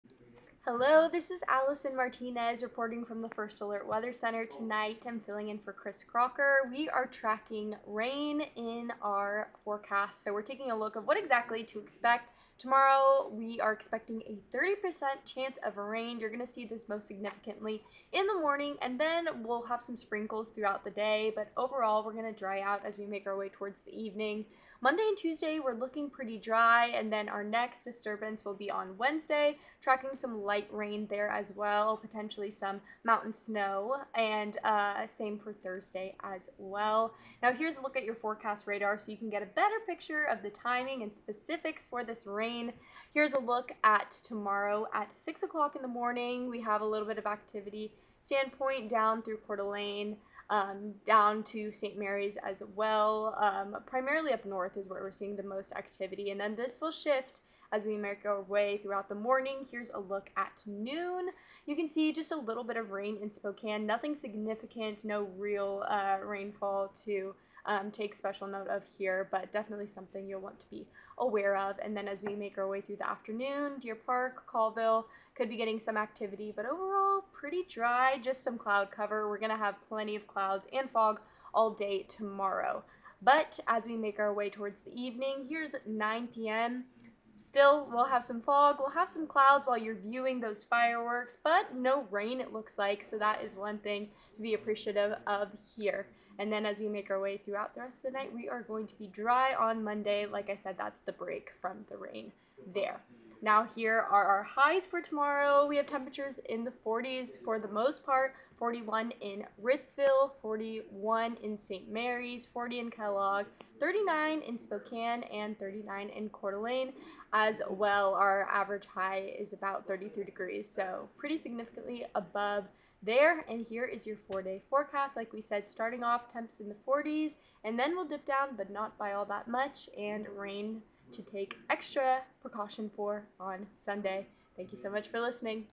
New Year's Eve rain: when to expect it
We're ending off 2023 with some light rain throughout the region. We're looking at a 30% chance of rain, with the most activity expected in the morning. Temperatures are warmer than average, so keep the heavy parkas at home, but bring out the hoods!

What We're Tracking
30% chance of rain tomorrow, mostly dry by night
Mild end to the New Year with warmer than average temperatures
Enjoy this weather before things cool down again late next week

Plan my Sunday (New Year's Eve)
Rain starting early Sunday morning
Clouds and fog may affect your firework viewing
Overnight lows expected to be ten degrees warmer than average!

A Closer Look
Temperatures in the upper 30s to 40s
Highest chance of rain Sunday, drying out for start of next week before rain returns again
Mild weekend overall